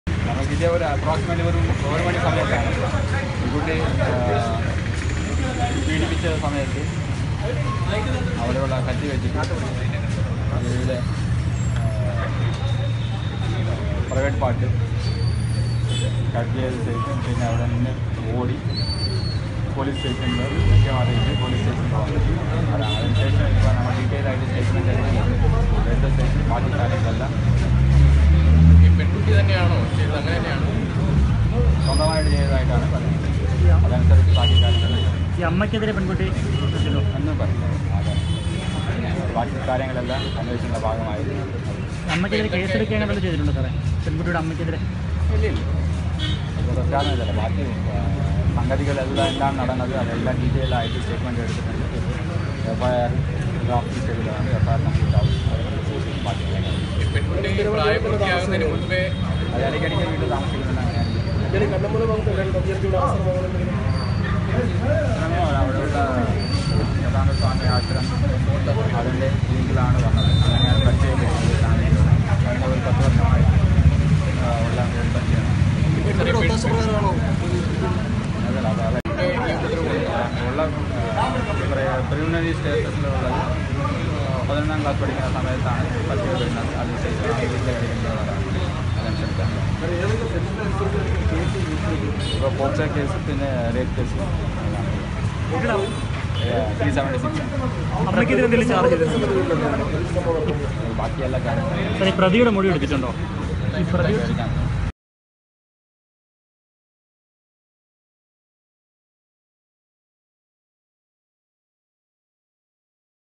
Pettah Rape Case: Commissioner Sparjan Kumar
Commissioner Sparjan Kumar talks to media about Pettah Rape Case at Thriuvananthapuram. Report:  Jithin SR.